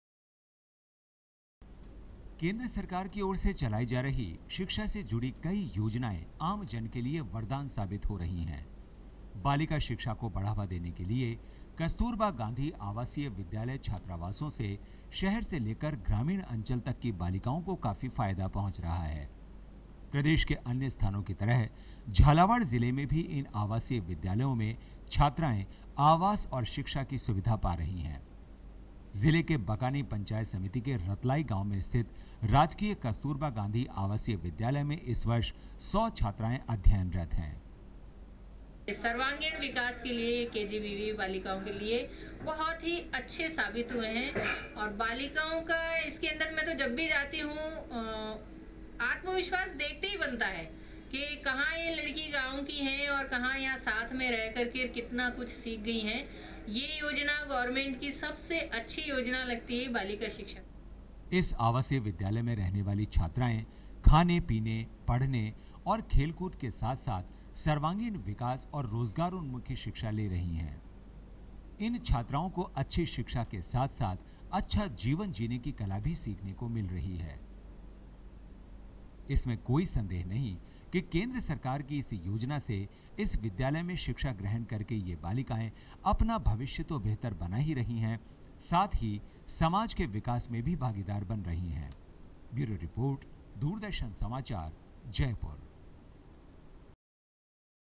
Kasturba Gandhi Balika Vidyalaya Scheme
The Government of India introduced the Kasturba Gandhi Balika Vidyalaya (KGBV) Scheme in August, 2004 to provide educational facilities for girls belonging to Scheduled Castes, Scheduled Tribes, Other Backward Classes, minority communities and families below the poverty line in Educationally Backward Blocks. The objective of KGBV is to ensure access and quality education to the girls of disadvantaged groups of society by setting up residential schools with boarding facilities at elementary level. The scheme has achieved phenomenal success in the field of girl education.